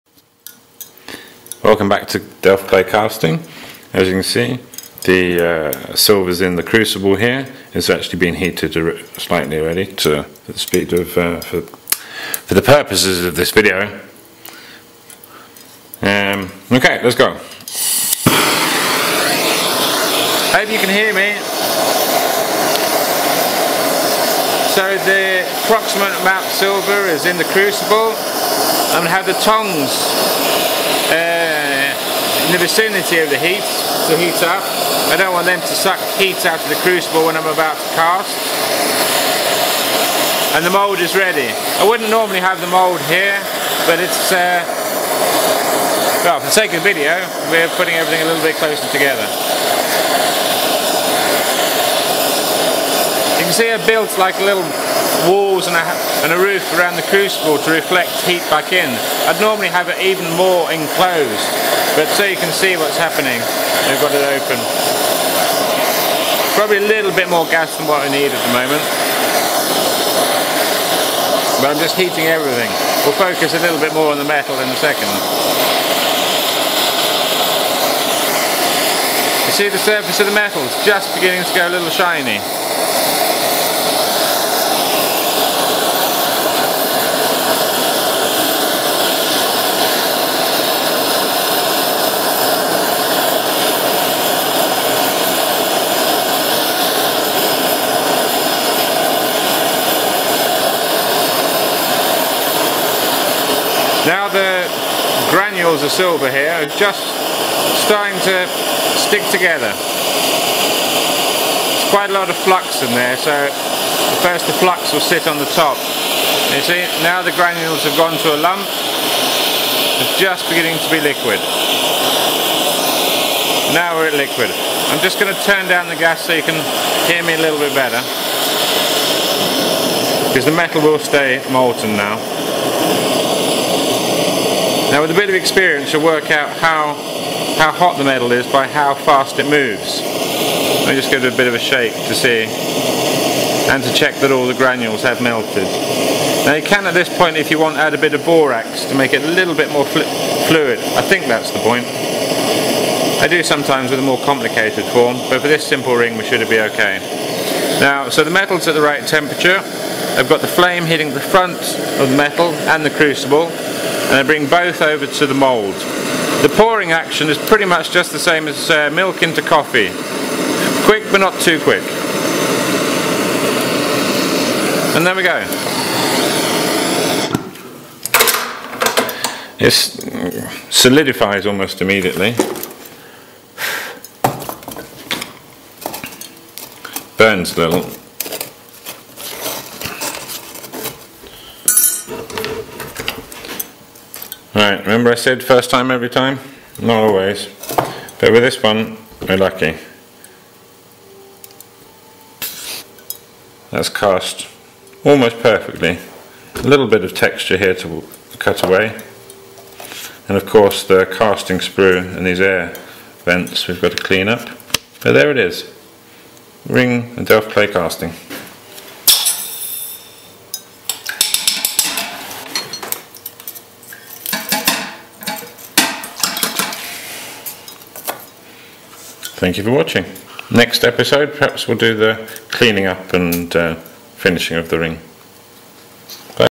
Casting silver in Delft clay-sand casting, pt 2.
This is my method for casting silver or yellow gold using Delft clay. I prefer to use a rectangular mold as they are more versatile and more successful.
Specifically, gravity is helping to a greater extent, the metal flows better into  all areas of the mold.

His ebay shop